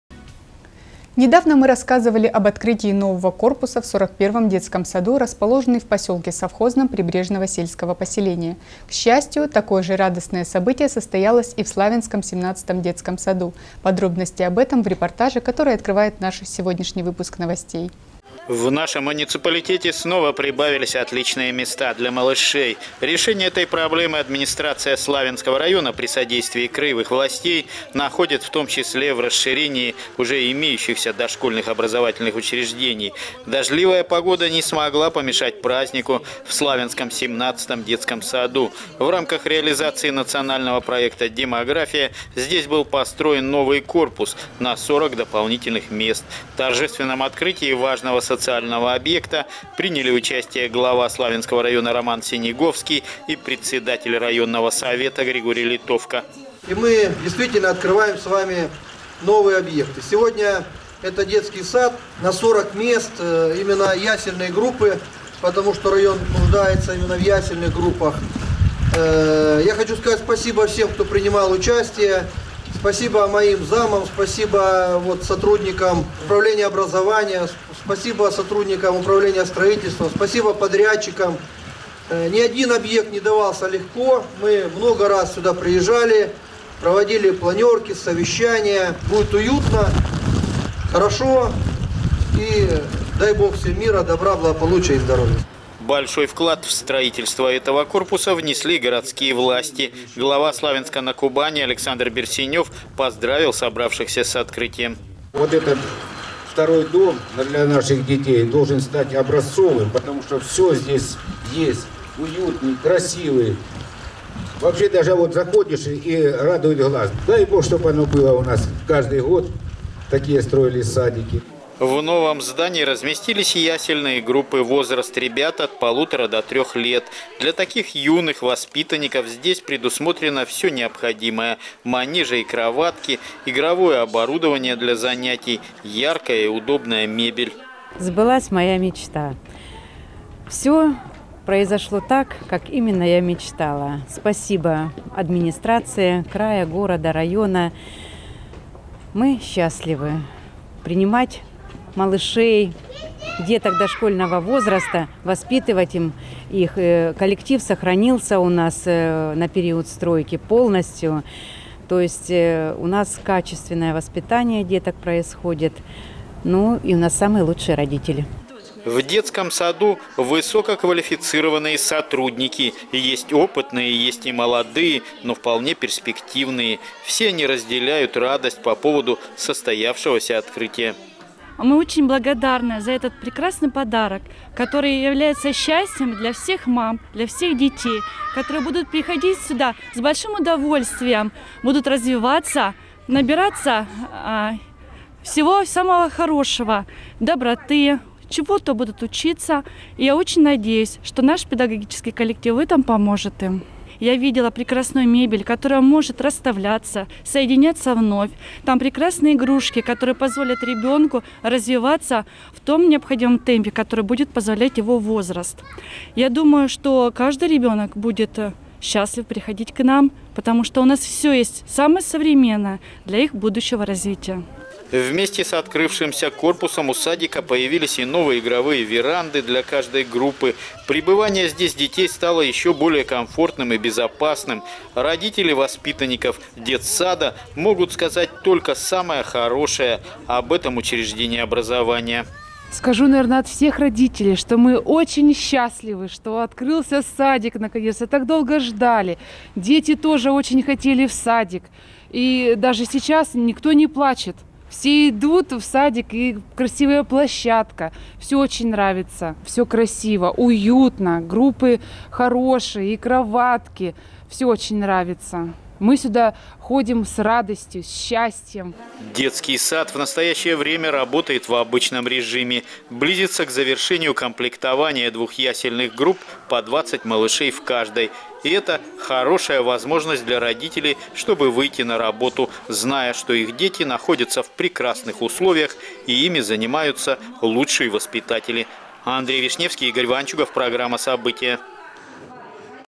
Одна большая радость на всех
Недавно мы рассказывали об открытии нового корпуса в сорок первом детском саду, расположенном   в поселке Совхозном  Прибрежного сельского поселения. К счастью, такое же радостное событие состоялось и в славянском семнадцатом детском саду